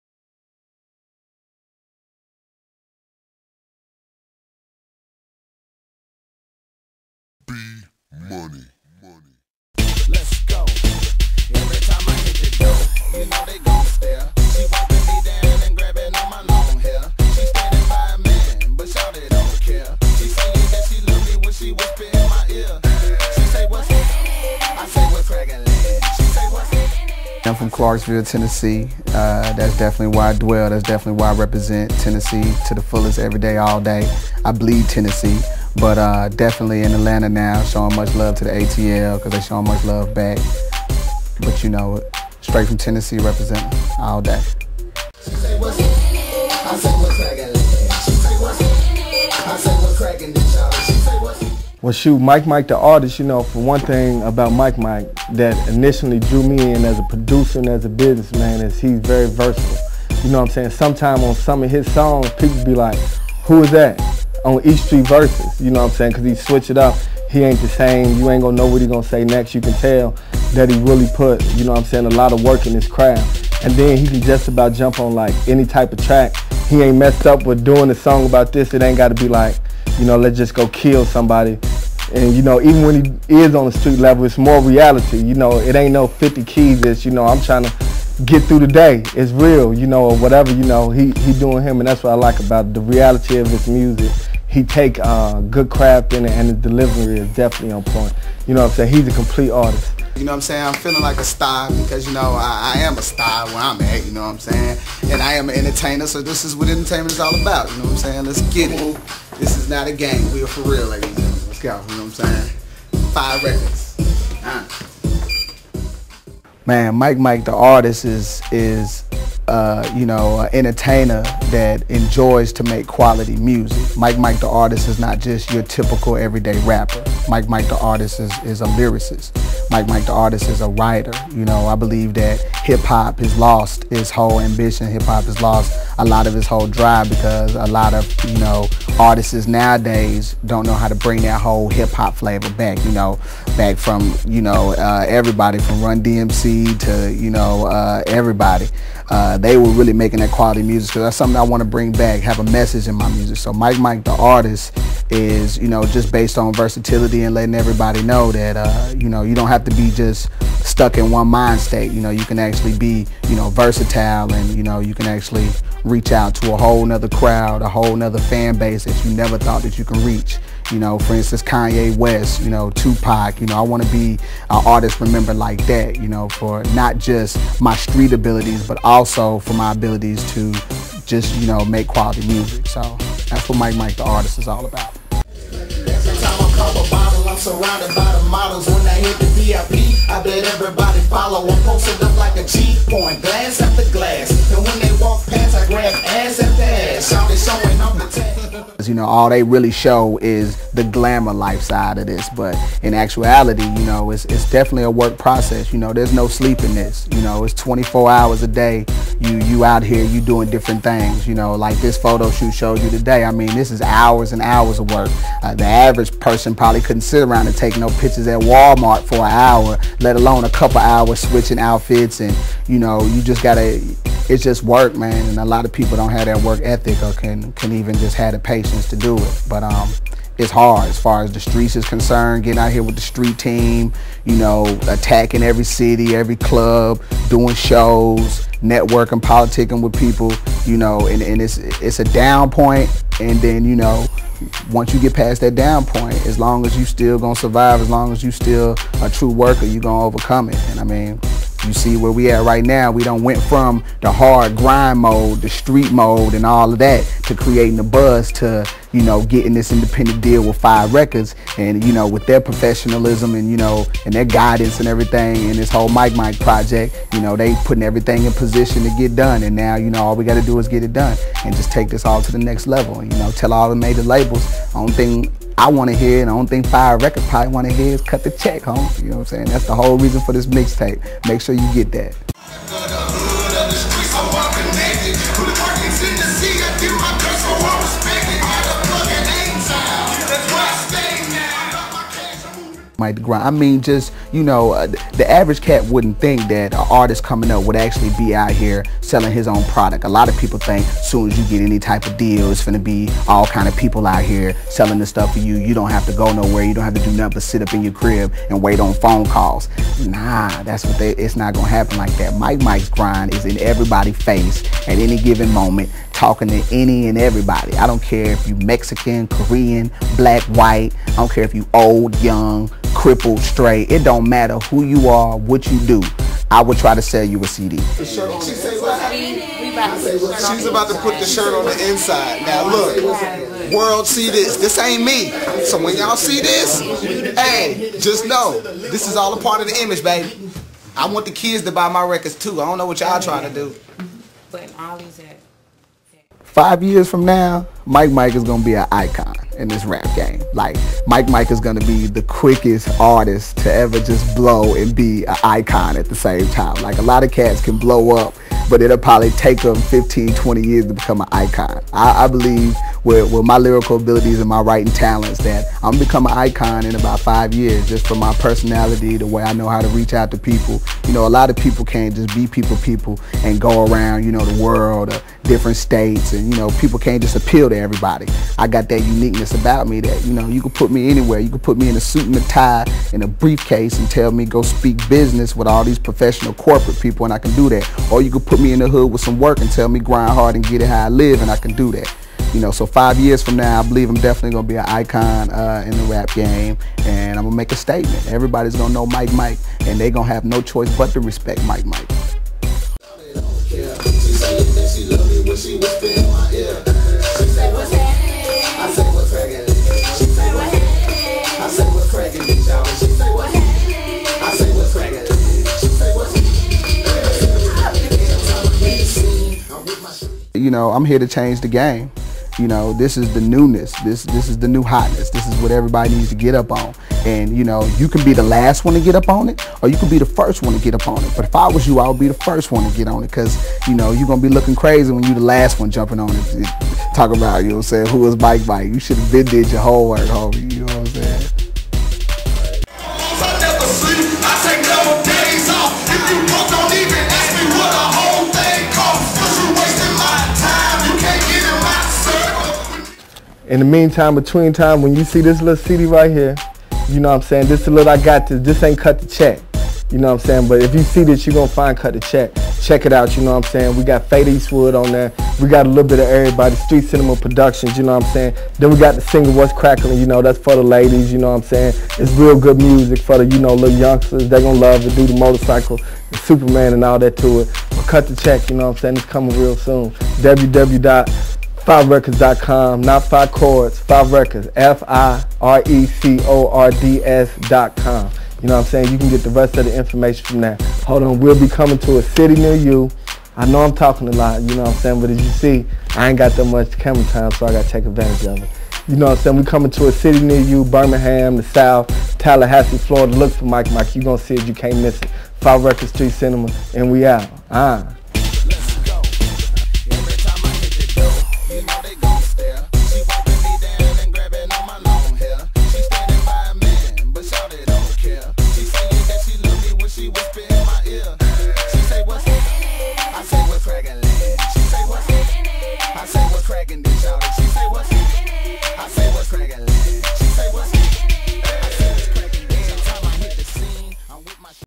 MikeMic Promo
Get a glimps of the rapper Mike Mic, the newest addition to the fi records family.  Video produced by Emmy Award Winning producer Rod Hollimon.